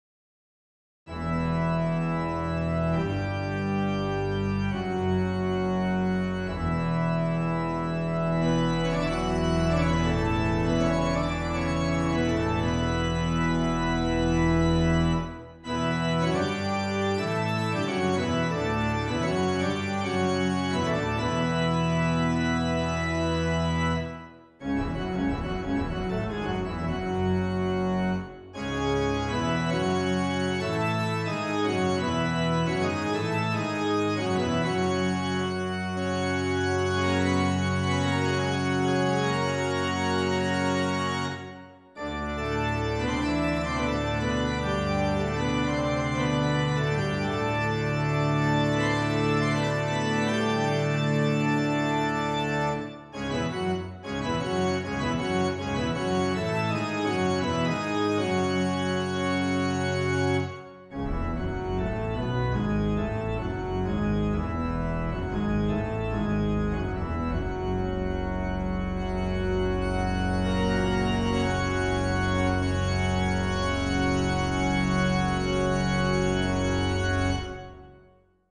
The Magic of Full Organ
Recently created a new custom organ (III/64), just wanted to showcase full organ. As long as you have the GrandOrgue software, and at least a couple of sample sets downloaded, you can use the following organ builder to create a custom organ.